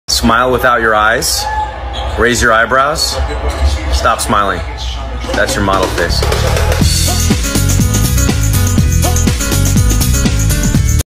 GRWM For my FIRST Photo Shoot! 📸
This was my FIRST EVER photo shoot and Definitely Won’t be the last! 👀
Had a lota fun behind the scenes as well! ☺️